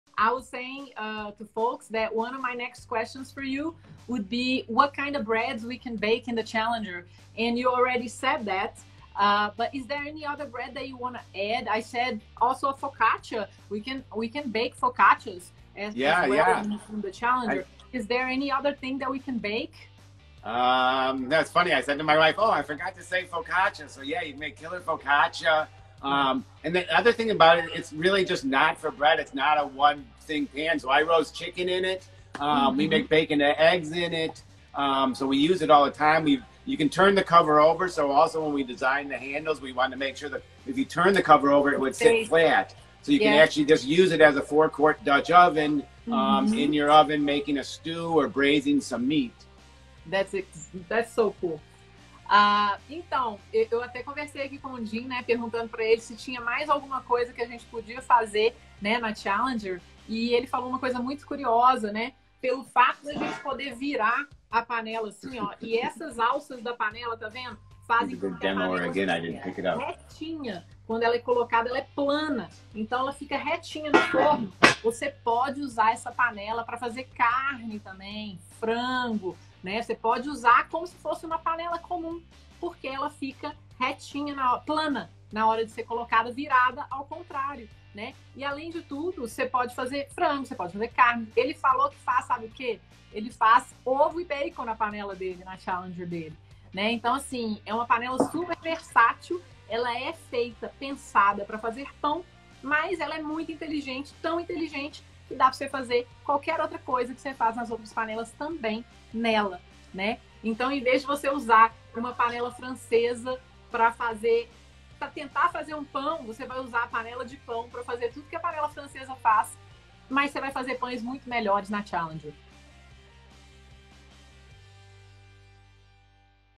O que você pode fazer na panela challeger?
Quer aprender dicas valiosas para produzir pães como esses no seu forno caseiro?
Clique no link abaixo e conheça todos os meus cursos 👇🏻

Olá, eu sou a Anna Riccio e amo fazer pães, especialmente os de fermentação natural. Nesse vídeo dou dicas rápidas testadas por mim, sobre pães de levain ou massa madre.

Se tiver alguma dúvida, é só escrever aqui nos comentários.

Porque o amor assa e fazer pão é massa. Isso é LOVE BAKES!

VOCÊ AINDA ME ENCONTRA NO...